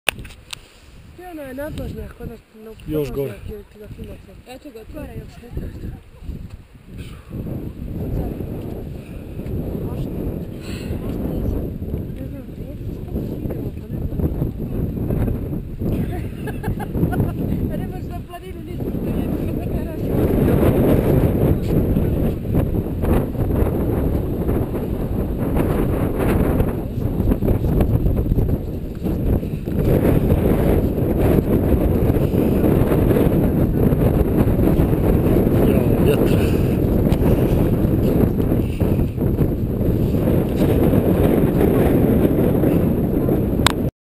Winter Olympic center on Jahorina mountain (part 04 of 10)
Short video taken on a visit to Winter Olympic ski center on Jahorina mountain (in Bosnia and Herzegovina, near Sarajevo).
The date was 2nd January 2012.